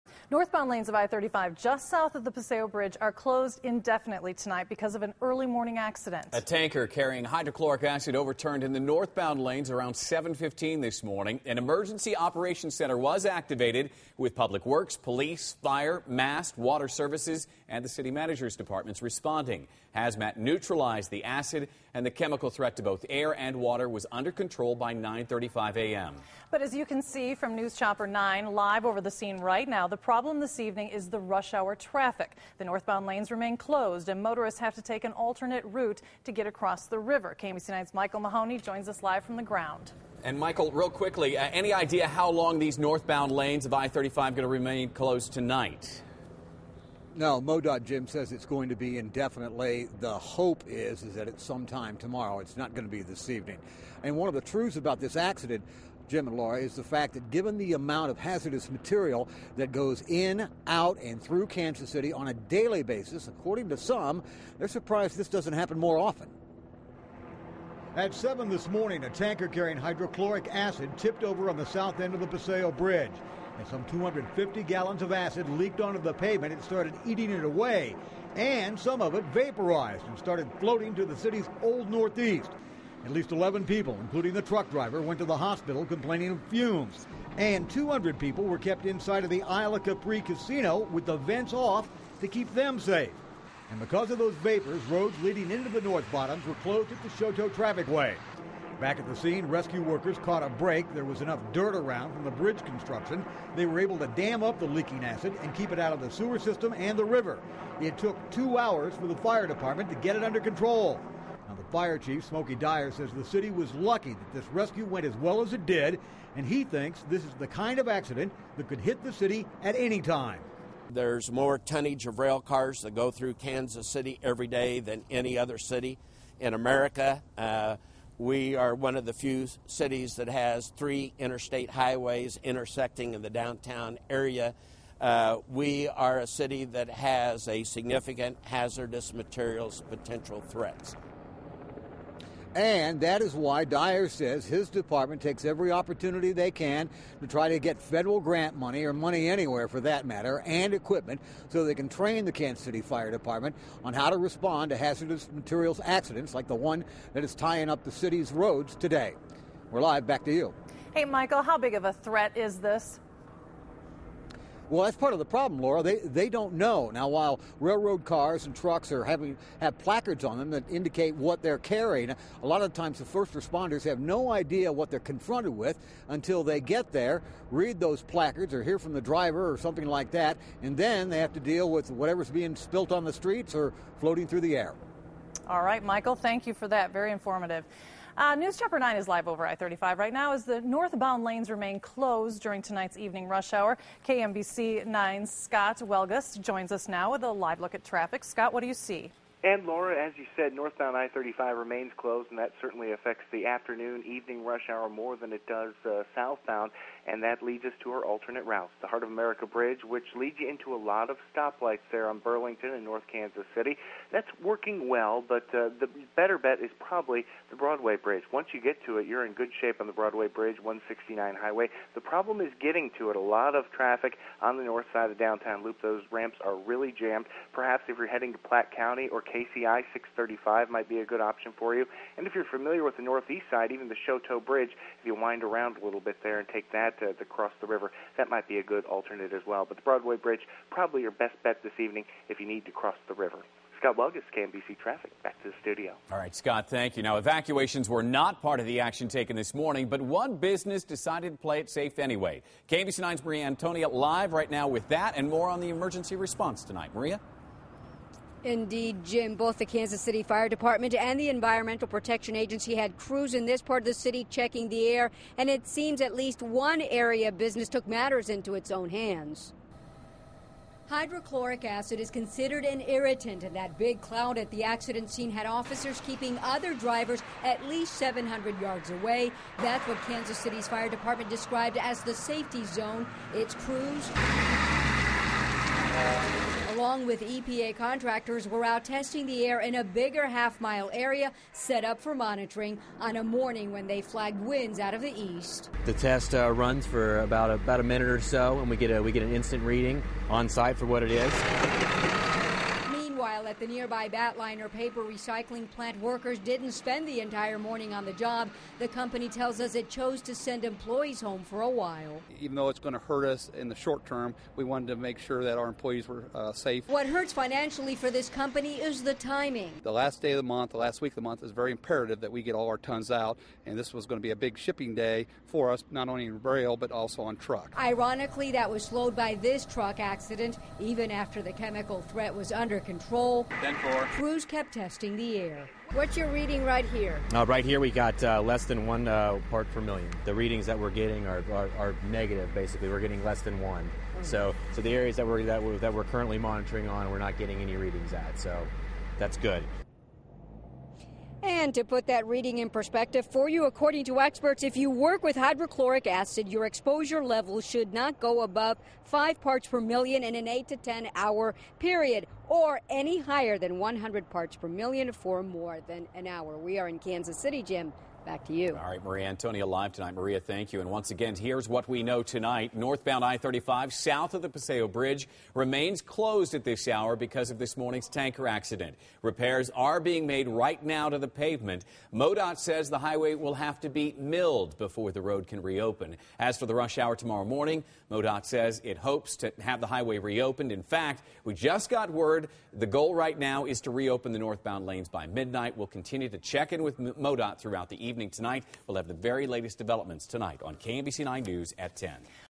Part Of I-35 Reopens Following Tanker Spill
The northbound lanes of Interstate 35 near the Paseo Bridge remain closed after a tractor-trailer overturned and spilled hydrochloric acid. KMBC's Micheal Mahoney and Maria Antonia report.